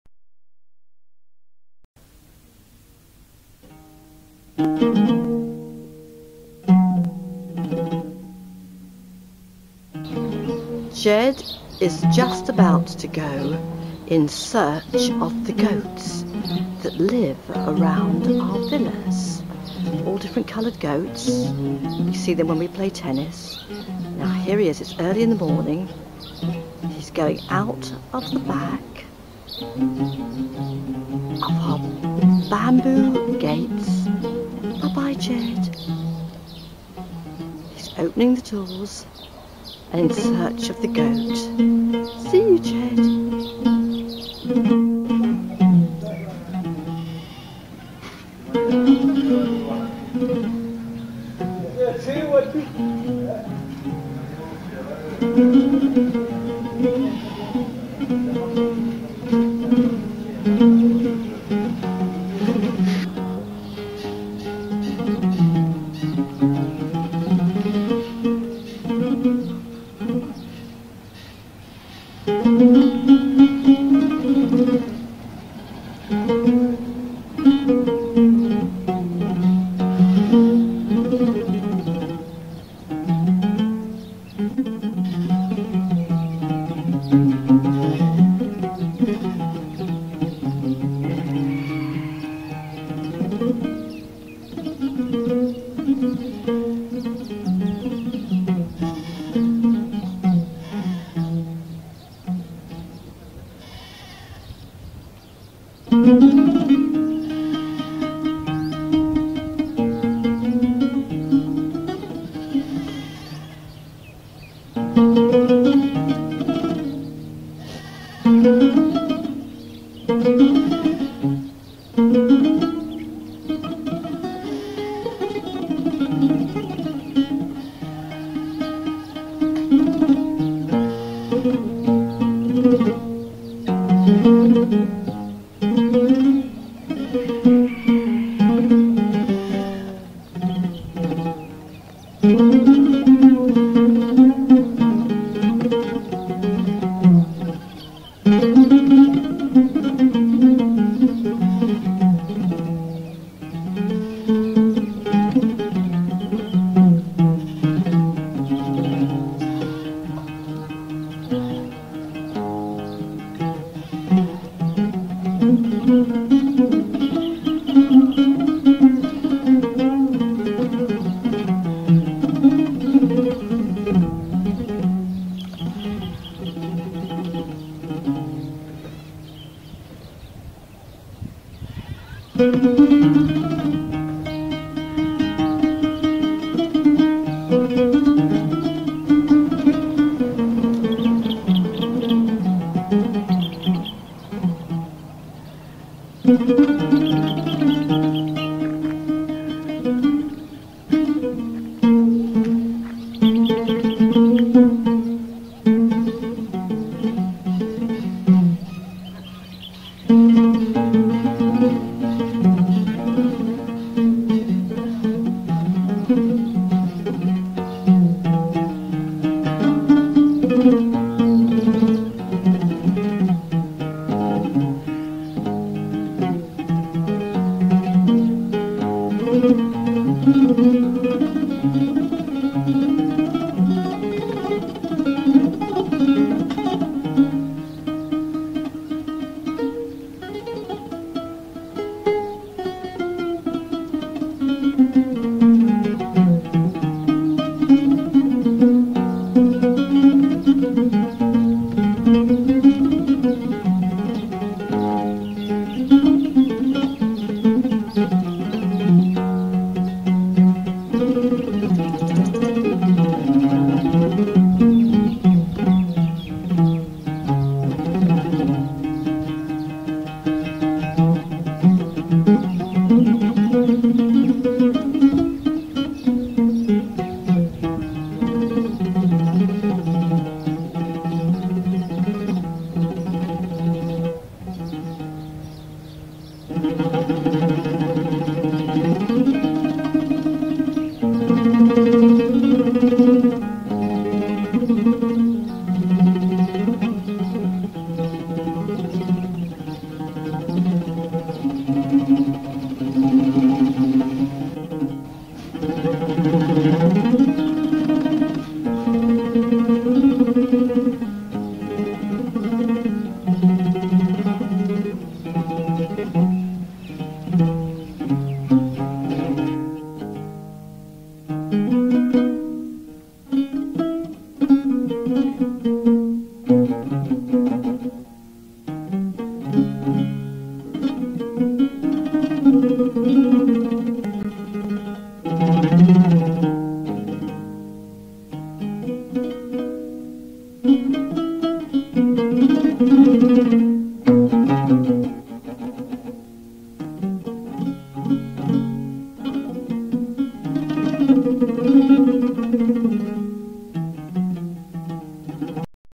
Ged & Daisy Discover Omani Goats
Ged goes off with his camera to discover the goats at Zighy Bay.  He succeeds in finding the goats, but the goatherd remains undiscovered.